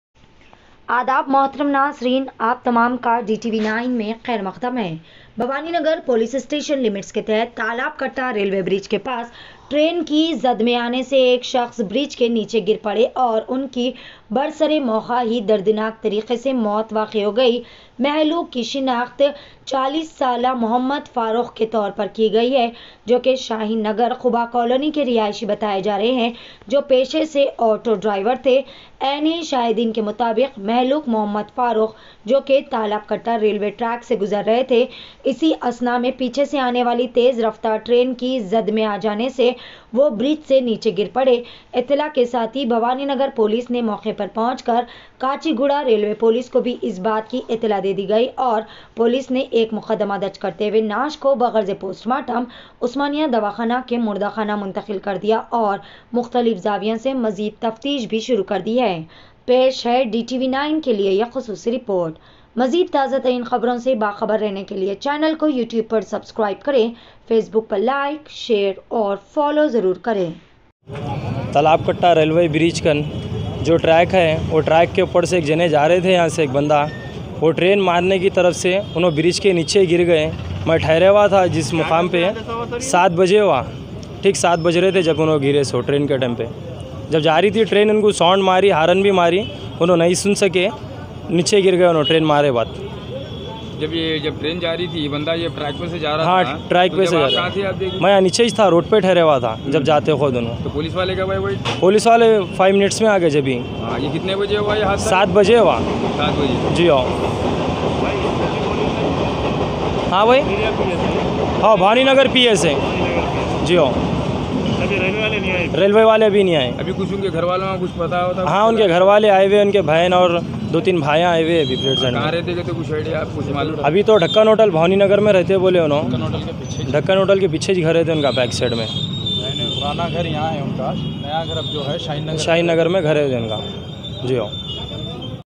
Railway Bridge se Guzarna Pada Mahenga | Mohammed Farooq Ko Train ne Maari Takkar.||DTV9
*Railway Bridge se Guzarna Pada Mahenga | Mohammed Farooq Ko Train ne Maari Takkar| Bridge se Neeche Girkar Shaqs Ki Hui Maut..!!*